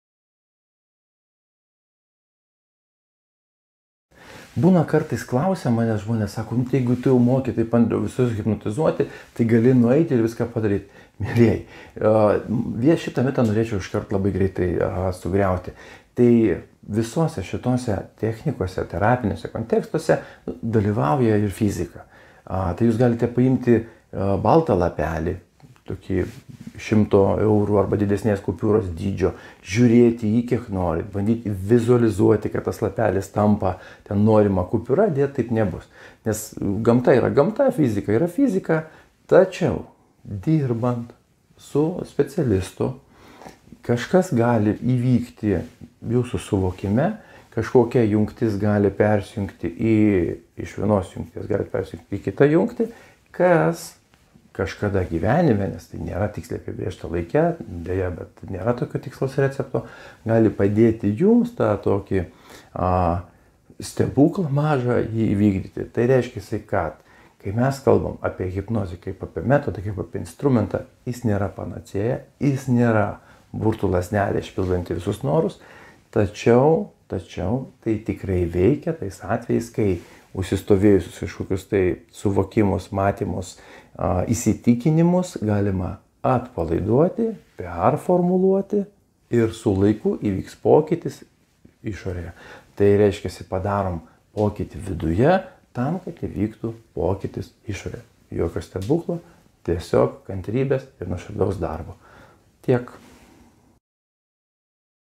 Mitas apie hipnozę kaip apie stebuklų lazdelę
Yra kas galvoja, kad hipnozė veikia kaip magija ar burtai. Tai visiškas mitas.
Gero žiūrėjimo.

Greitesnei komunikacijai:
Andrius Jarašiūnas